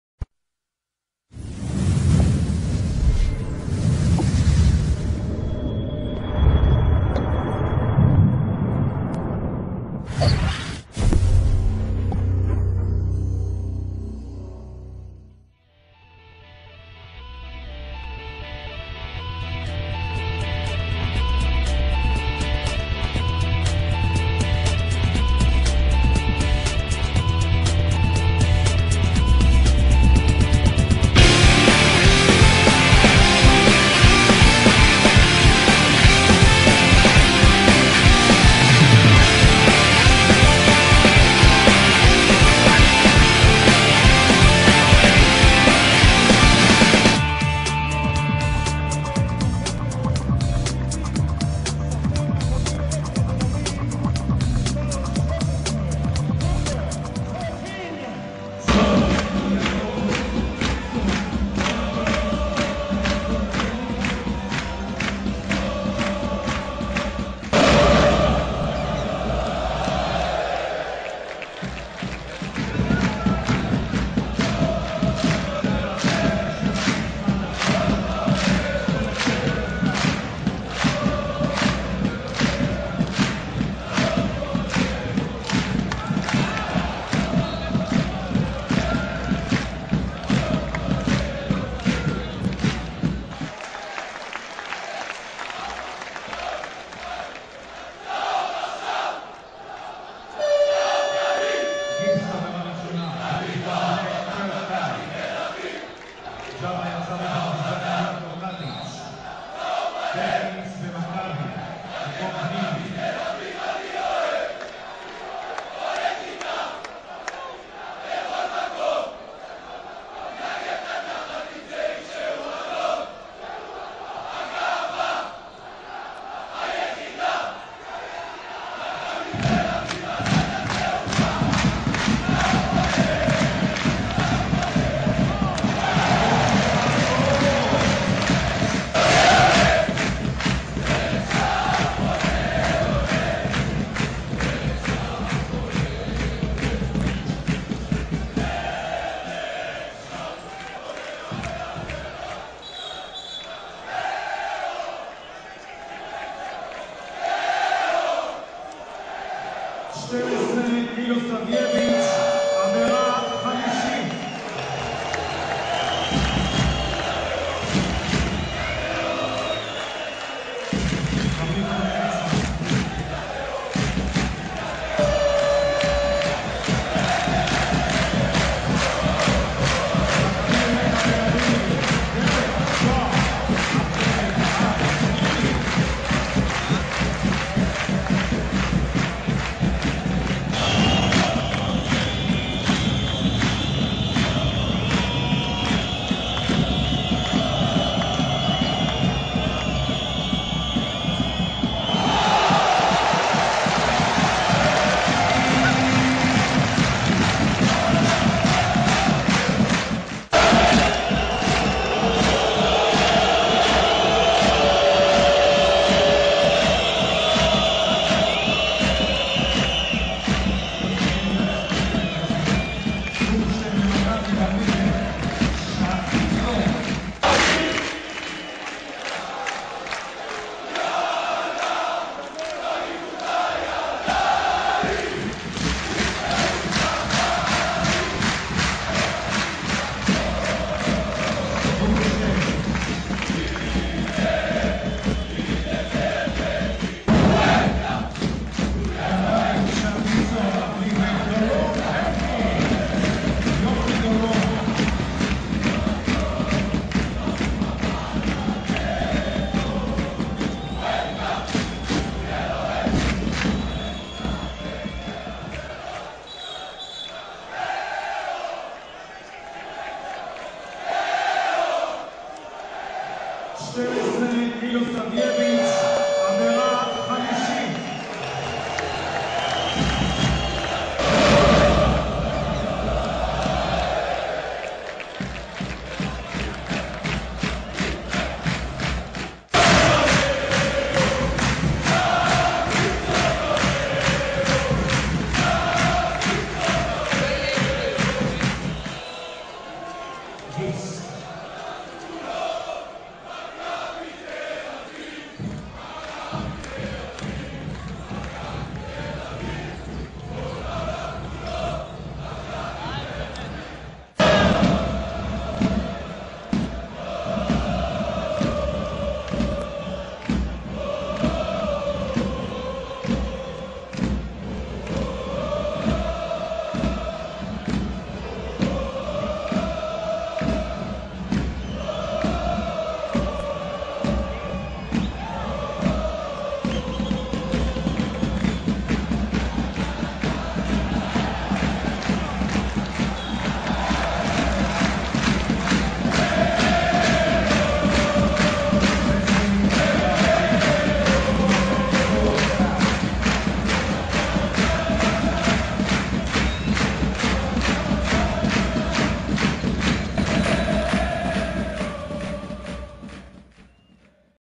GATE 11 - MACCABI Vs. Partizan 9.12.10.avi
GATE 11, Maccabi Tel Aviv fanatic fans in the team's Euroleague match against Partizan mt:s on the 9/12/2010.
Displaying the TIFO supporting all of the people hurt in the big wildfire that happened in the north of the country of israel and chants supporting the team durring the game.

Final score: Maccabi Tel AVIV - 76- 62 - Partizan mt:s

שער 11, אוהדי מכבי ת"א במשחק היורוליג של הקבוצה נגד פרטיזן בלגרד בתאריך 9/12/2010.
מציגים את התפאורת תמיכה בתושבי הצפון שנפגעו במהלך השריפה בכרמל וקטעי עידוד מהמשחק עצמו

תוצאת המשחק: מכבי 76 -62 פרטיזן בלגרד.

GATE11.co.il - אתר אוהדי מכבי תל אביב, שער 11
GATE11.co.il/photo - גלריית האתר של שער 11
GATE11.co.il/forum - פורום האוהדים הרישמי של שער 11